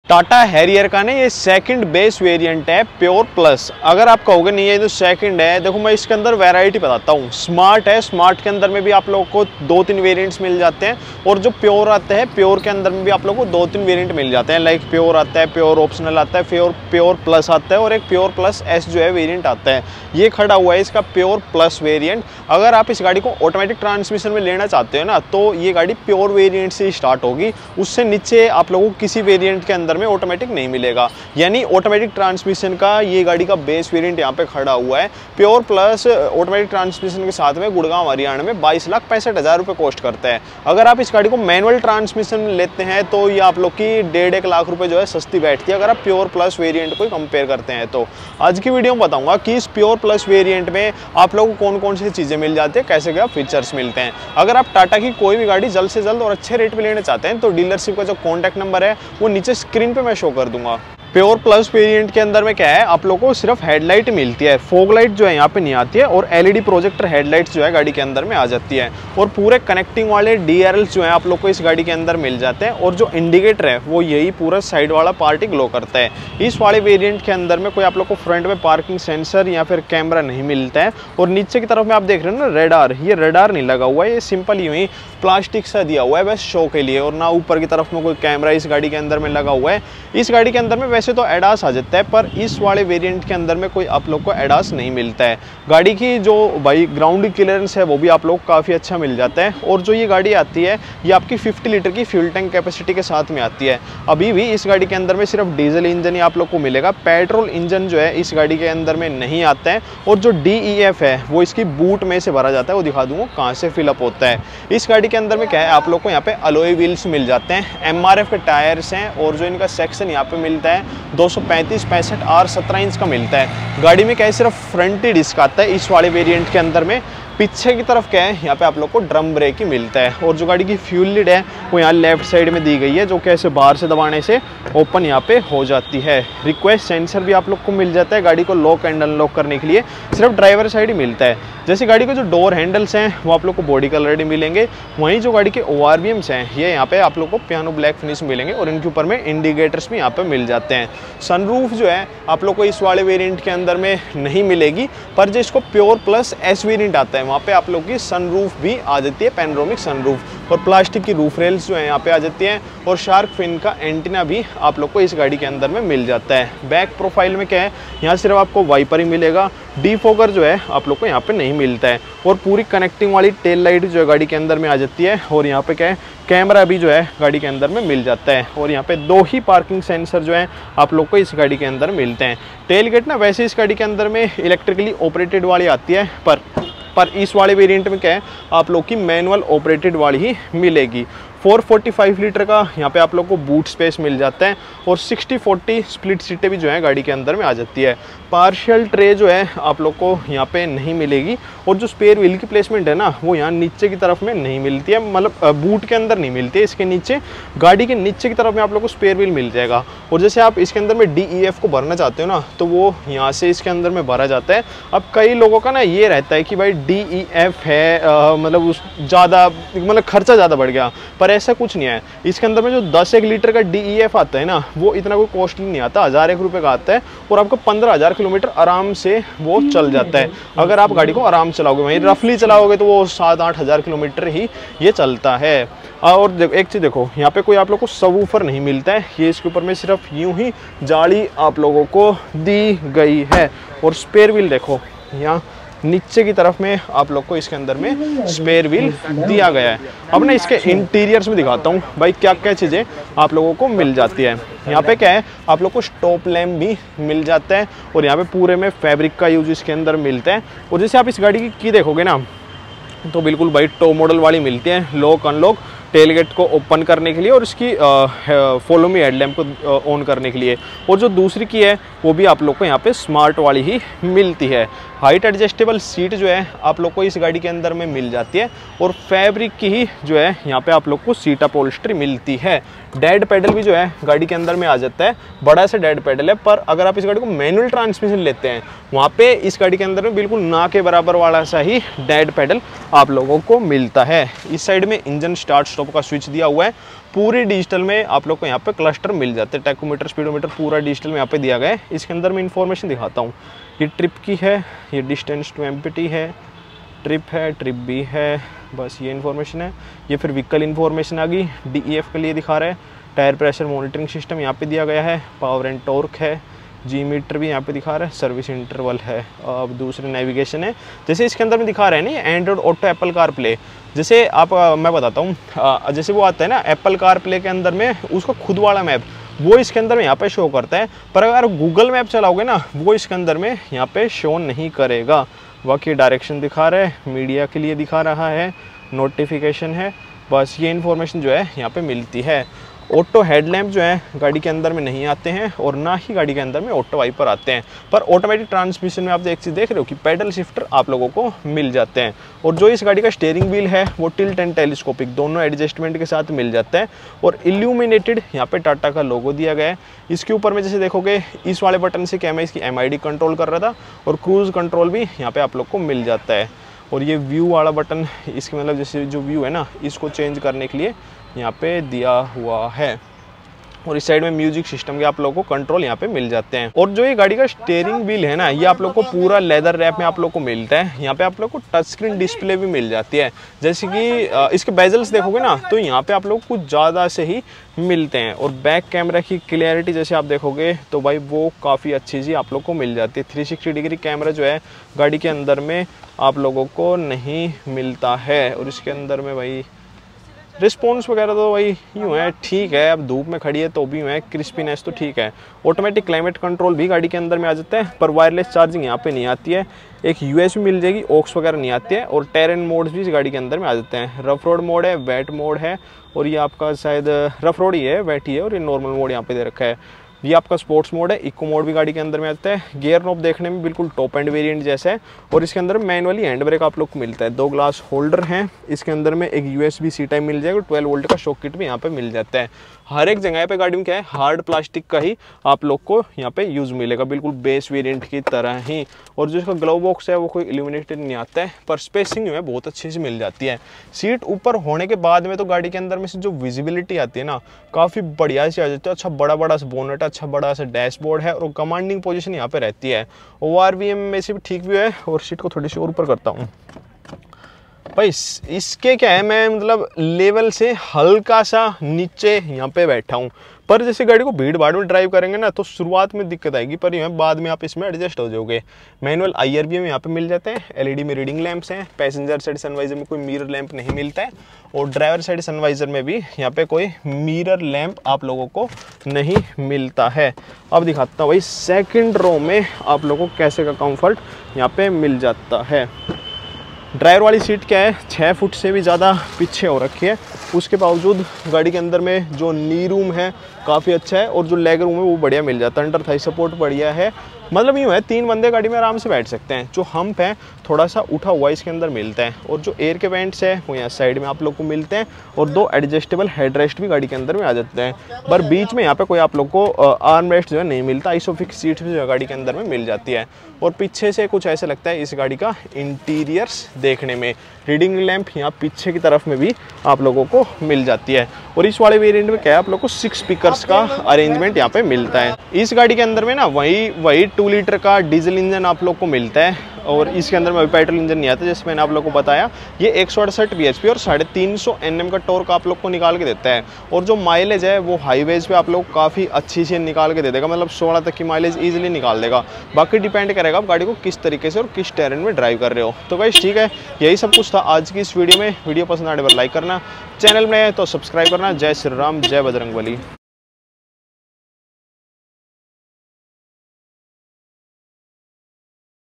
*2nd Base & All Features* New Tata Harrier Pure+ is Finally Here! Review
Watch in this video-*2nd Base & All Features* New Tata Harrier Pure+ is Finally Here! Review

If you want to share your ownership Experience,